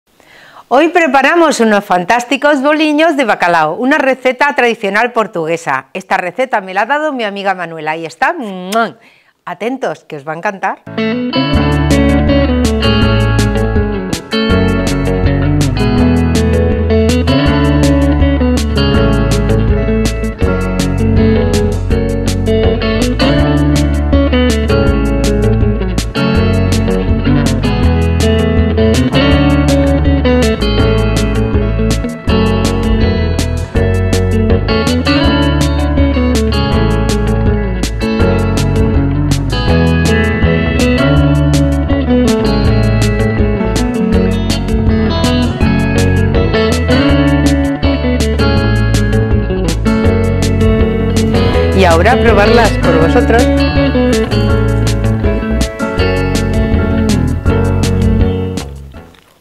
👆LA RECETA 👆 COMPLETA 👆 PINCHANDO ARRIBA
BOLINHOS DE BACALHAU RECETA TRADICIONAL PORTUGUESA. Deliciosa receta tradicional portuguesa que se hace de manera fácil, la mezcla de patatas y bacalao es riquísima y es un aperitivo o entrante genial especialmente en Semana santa, cuando por tradición el bacalao está presente en nuestra mesa, aunque son una delicia en cualquier época del año estos bolinhos de bacalhau receta tradicional portuguesa.
Ingredientes de los BOLINHOS DE BACALHAU RECETA TRADICIONAL:
700g de bacalao
700g de patatas peladas
1/2 cebolla
Unas ramitas de perejil
1 ajo
Pimienta recién molida (Cantidad al gusto)
Sal (Cantidad al gusto)
3 huevos de tamaño grande